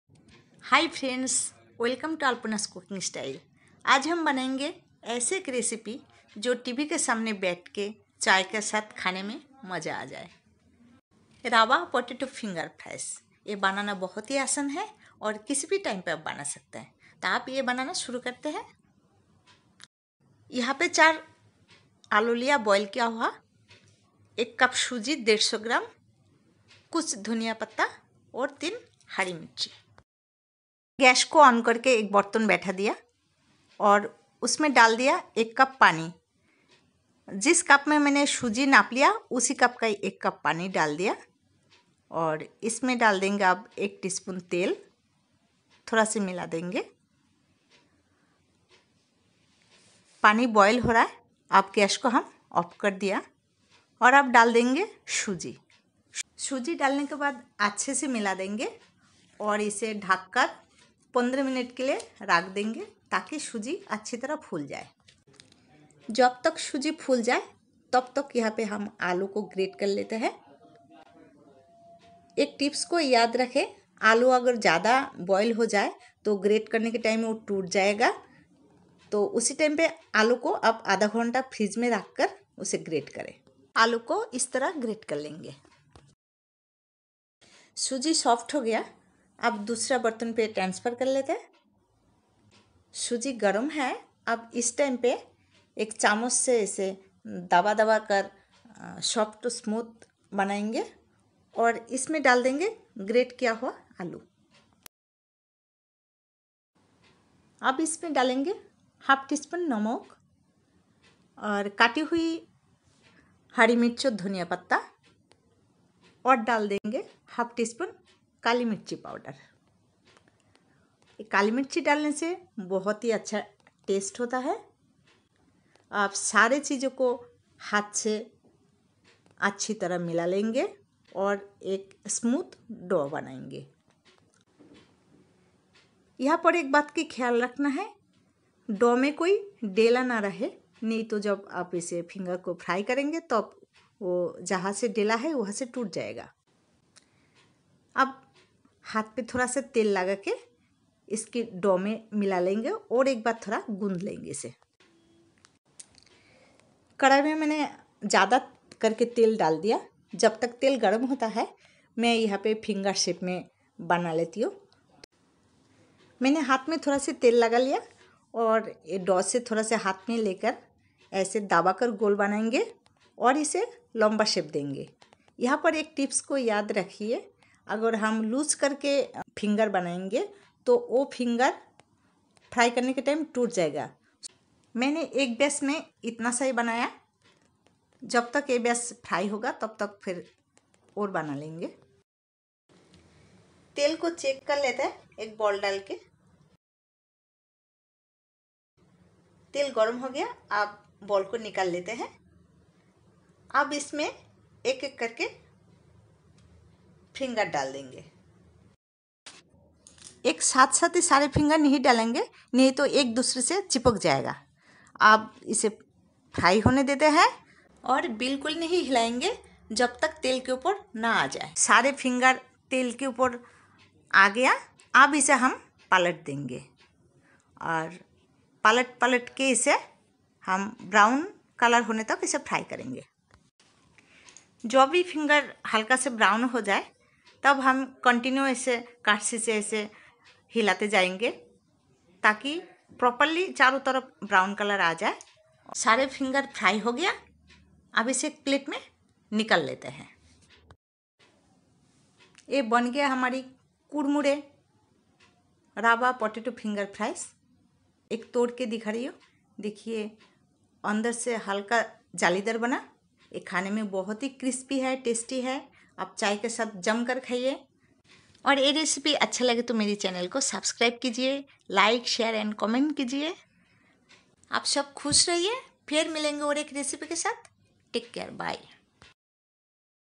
How to make perfect Rava Potato Finger Fries with Tips and Tricks | Potato Finger Fries Recipe
A lip-smacking food prepared mainly from boiled potatoes, semolina, coriander leaves. It is an ideal party starter and can be easily prepared

Ingredients :

Potatoes, Semolina, Coriander leaves, Green chillies, Black pepper, Oil and Salt